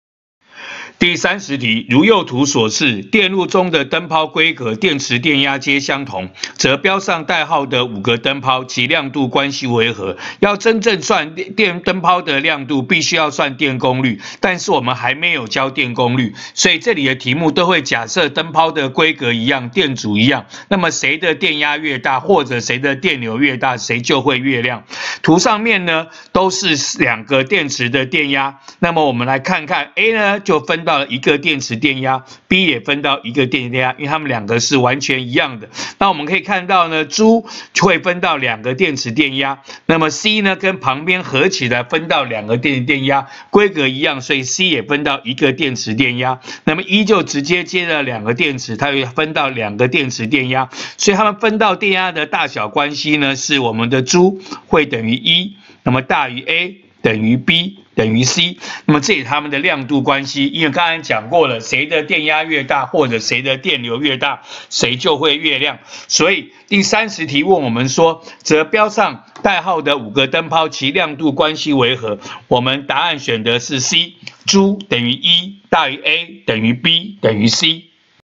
107學年上9年級理化三段第30題
三個電路，五個燈泡，問亮度關係。